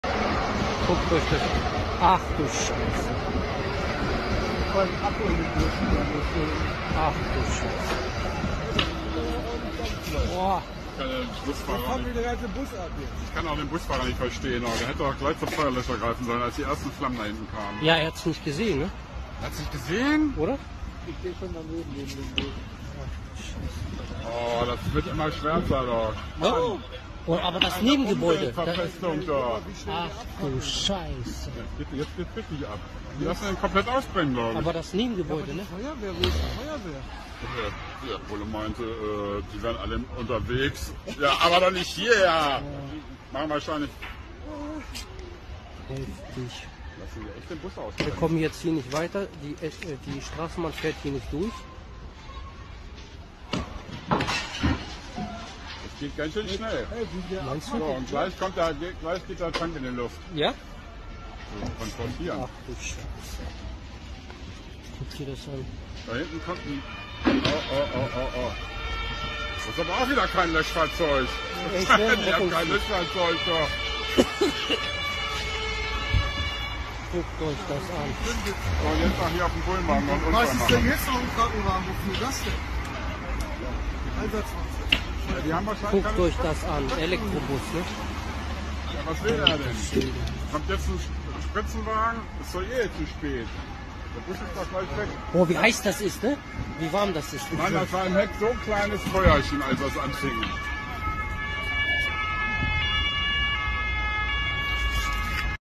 Elektrobus Brand in Hannover. Januar 2023. Netzfund
Hier brennt ein Vertreter der Erfolgsgeschichte Elektromobilität. Ich wage eine Prognose. Das wird sich alles in Wohlgefallen Auflösen…In ein paar Jahren, wetten?😉